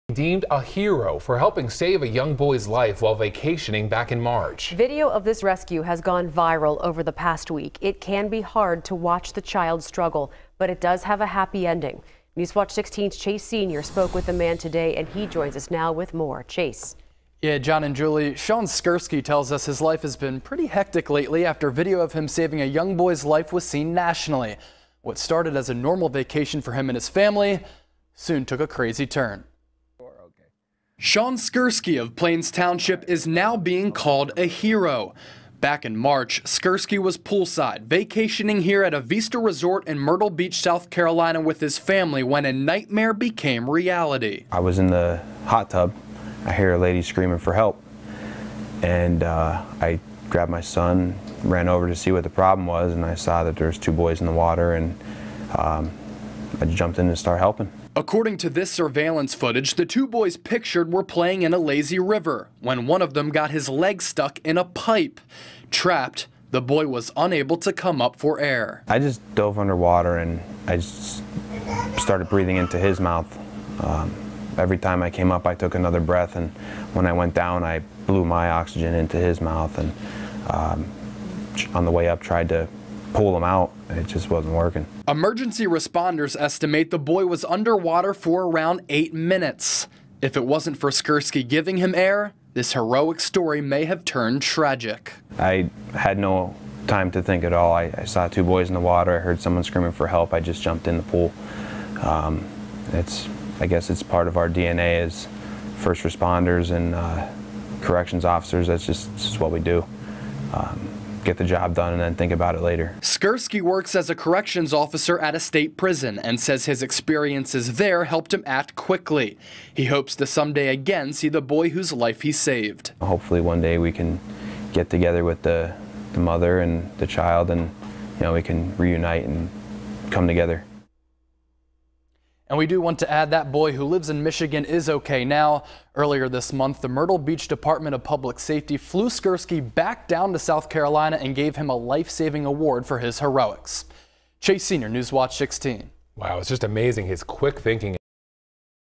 Vacation Hero: Luzerne County Man Saves Boy from Drowning
One man from Luzerne County is being deemed a hero for saving a young boy's life while vacationing back in March.

Shaun Skursky tells Newswatch 16 his life has been pretty hectic lately after video of him saving a young boy's life gained national attention. What started as a normal vacation for him and his family took a crazy turn.